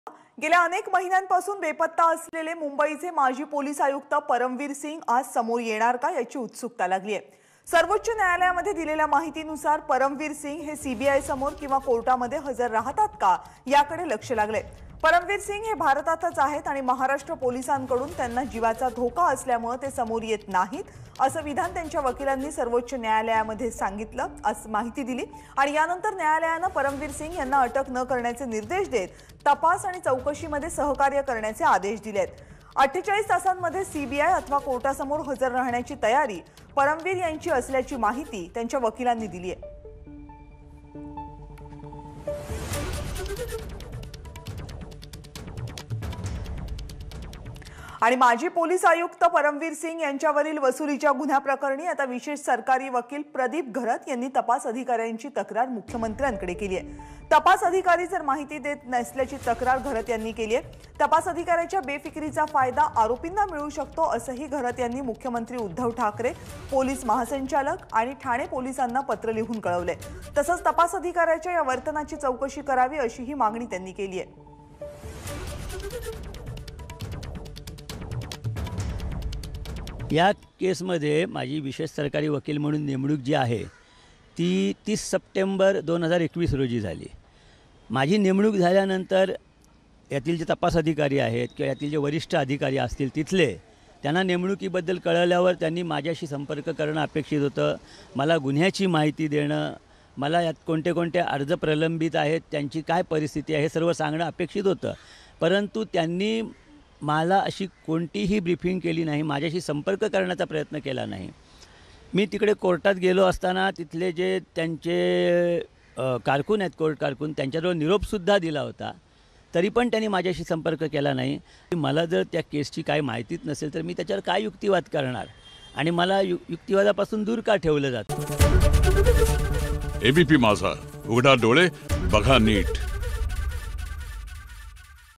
Param Bir Singh Case बाबत तपास अधिकारी माहिती देत नसल्याची सरकारी वकीलांची मुख्यमंत्र्यांकडे तक्रार
ABPMajha Param Bir Singh Case बाबत तपास अधिकारी माहिती देत नसल्याची सरकारी वकीलांची मुख्यमंत्र्यांकडे तक्रार